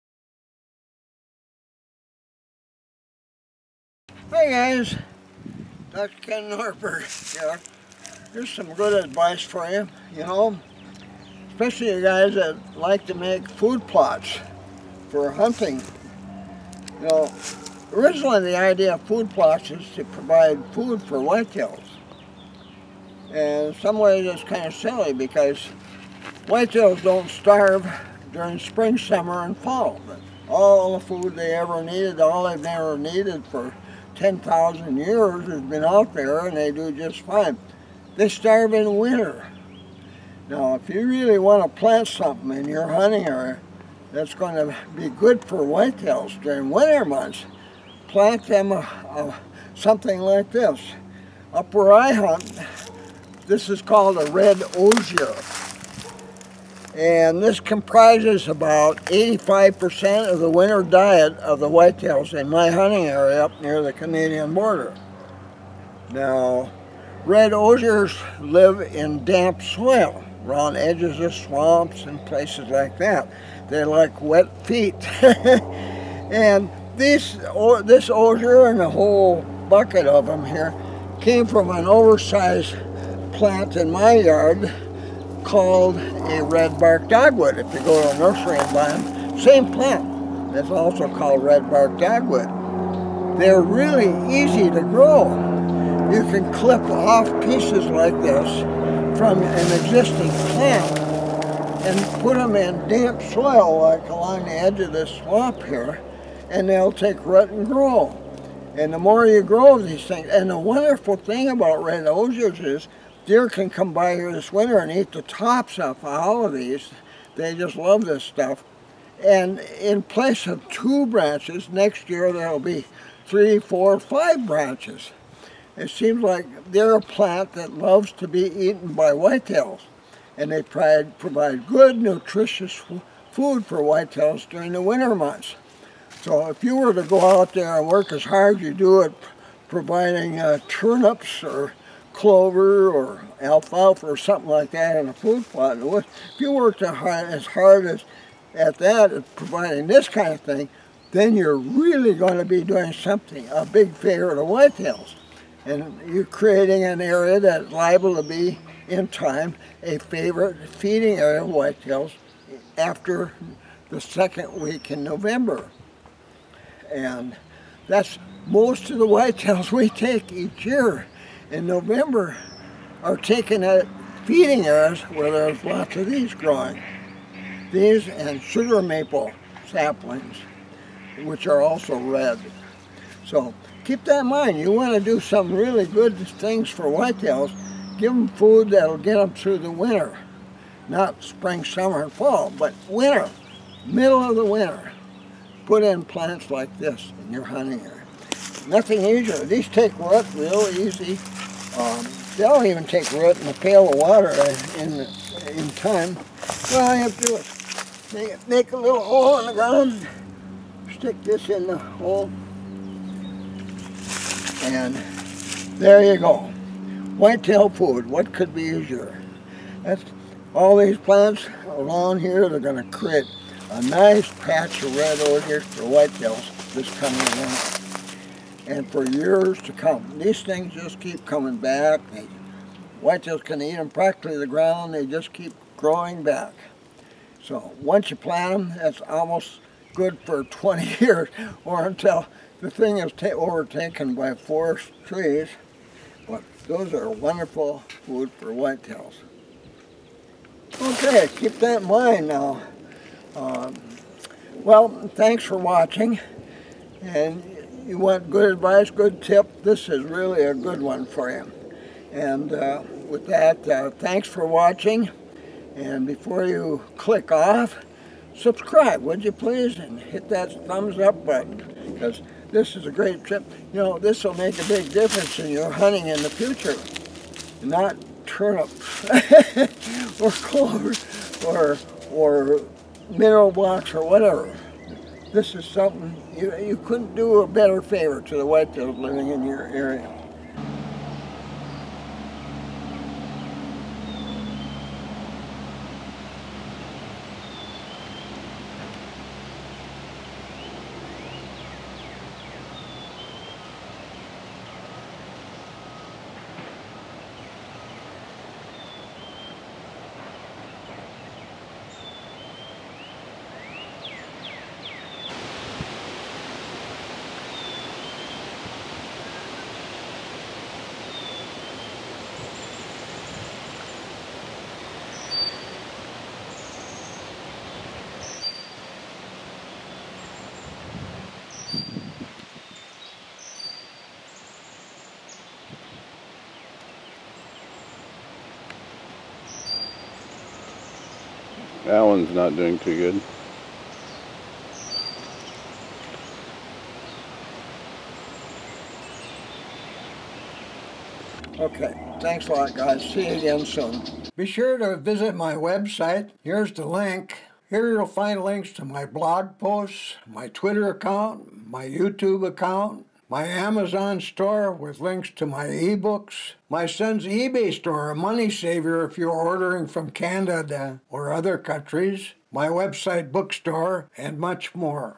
Deer Hunters Help Your Whitetails by Growing Red Osier (Winter Browse Plants)
There are many deer hunters out there that are growing food plots to feed their deer. Mostly, they plant foods deer would eat in the summer and fall. However, deer do not need help during these periods. In the winter, starting about the first week in November, deer switch to eating browse foods. In northern states Red Osier, or Red-Barked Dogwood, makes up a significant portion of their diet. If you want to help whitetail deer with food that will help them in winter months, plant browse plants, like Red Osier.